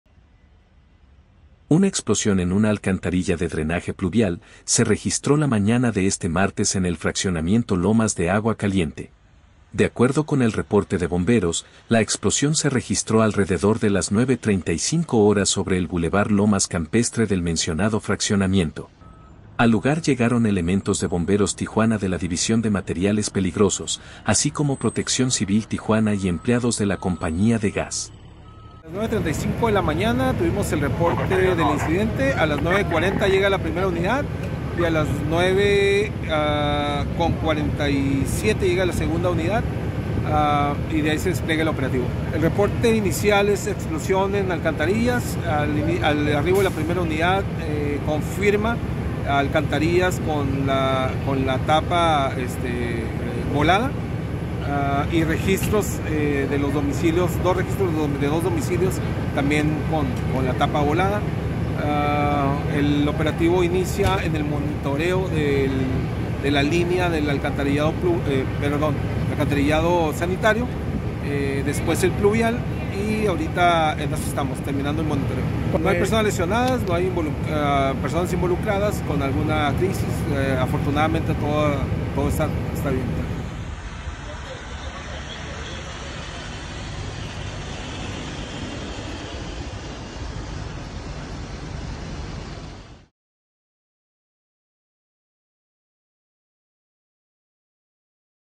Se registra explosión de alcantarillado en Tijuana
Tijuana | Se registra explosión de alcantarillado en Lomas de Agua Caliente. 🚒

De acuerdo con el reporte de Bomberos, la explosión se registró alrededor de las 9:35 horas sobre el bulevar Lomas Campestre del mencionado fraccionamiento.

📹 Sergio Ortíz / Cortesía